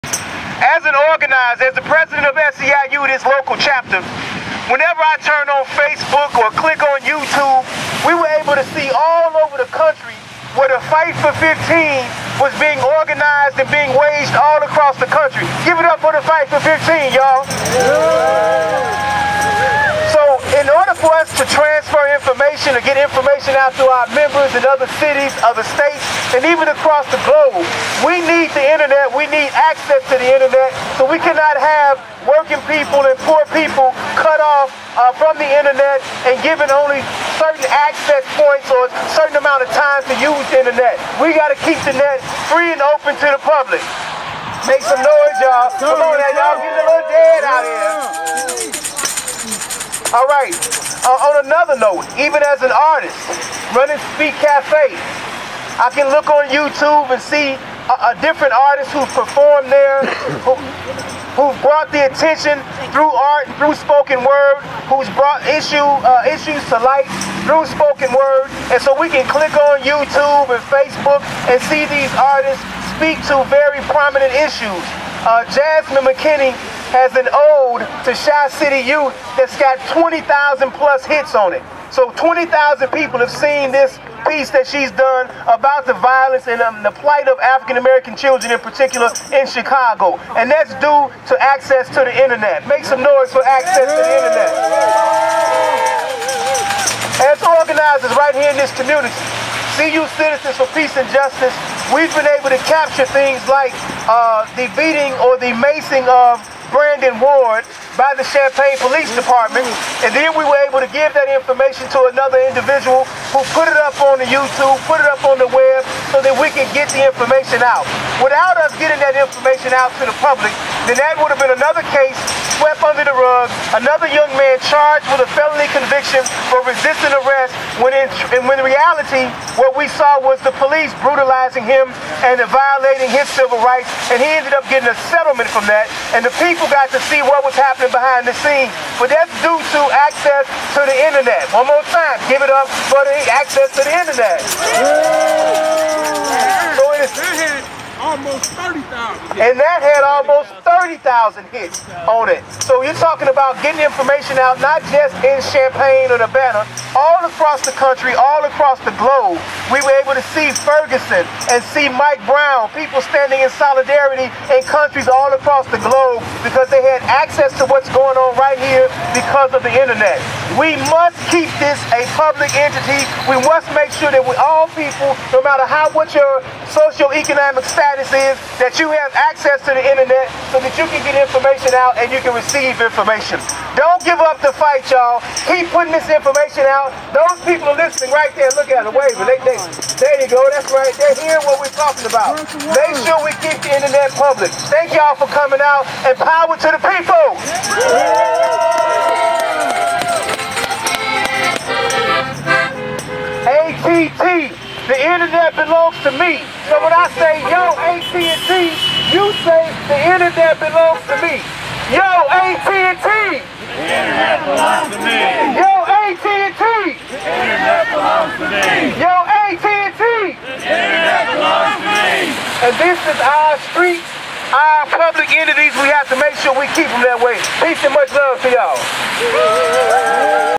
Aaron Ammons Speaking Out at the DontBlockMyInternet Rally in Champaign Feb 21, 2015
Aaron Ammons, Urbana City Council Member, President of Chapter 119 of SEIU Local 73 and Co-Founder of the local civil rights group Champaign Urbana Citizens for Peace and Justice speaks out for why we need an open internet.